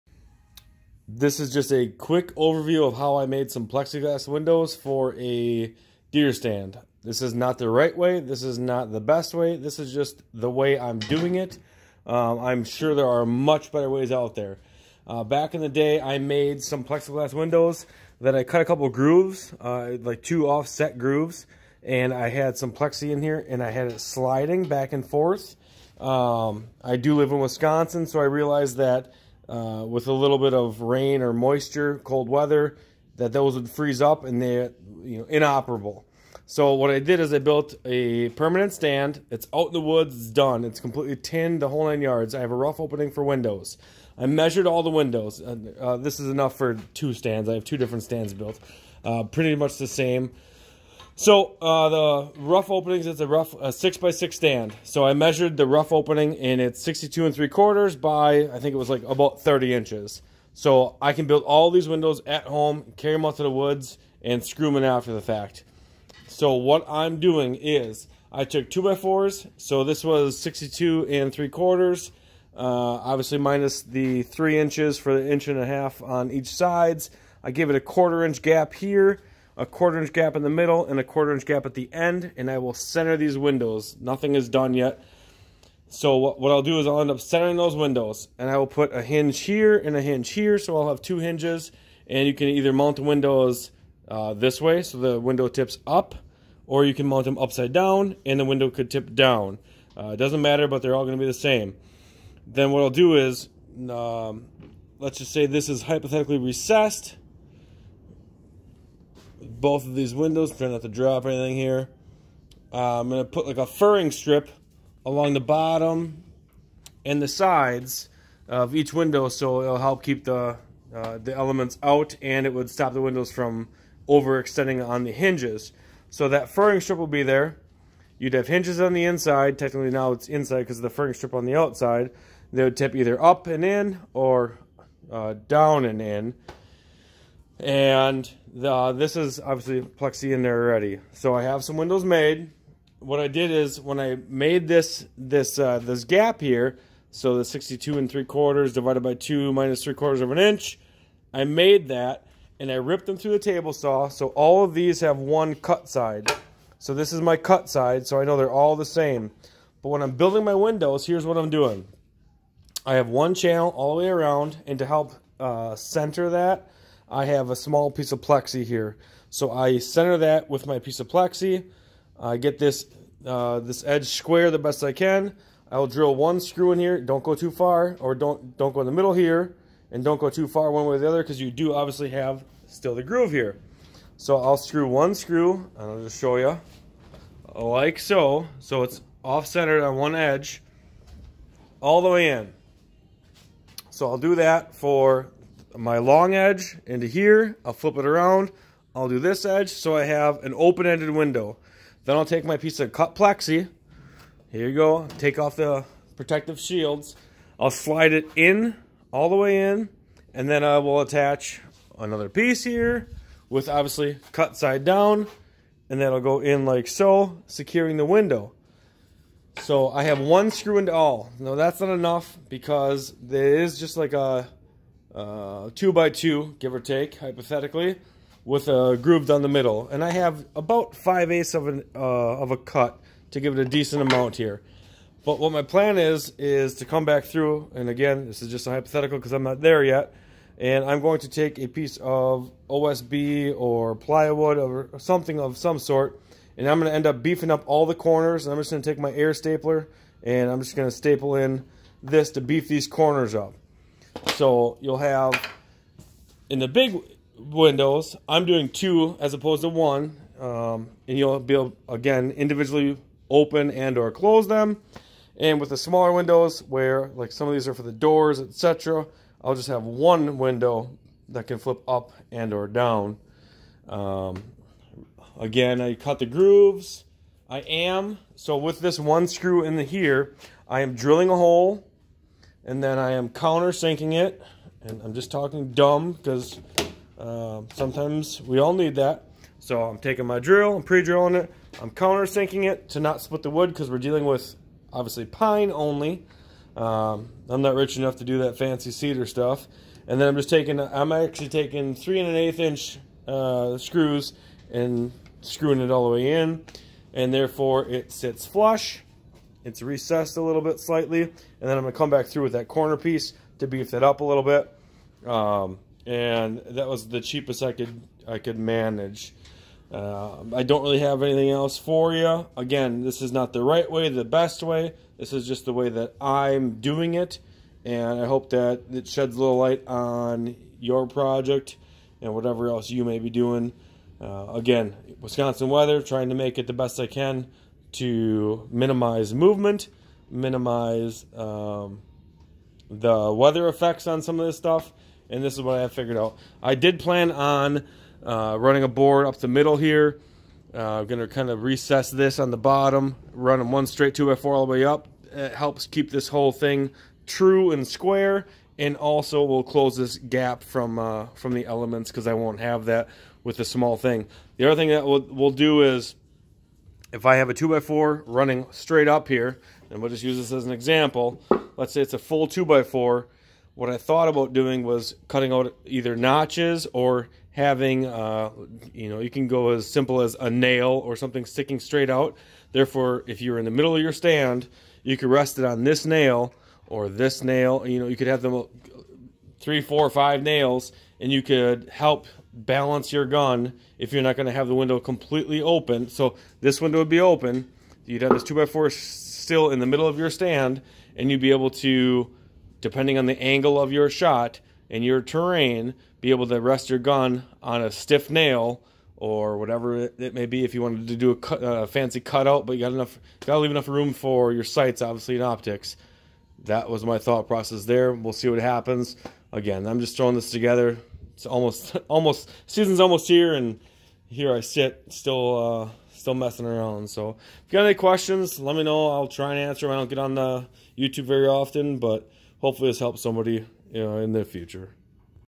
How to Make Plexiglass windows deer stand blind
Finished product in other video. Quick overview of what I did. I'm sure not the best way or right way but it's what I threw together. Just want to give some people ideas.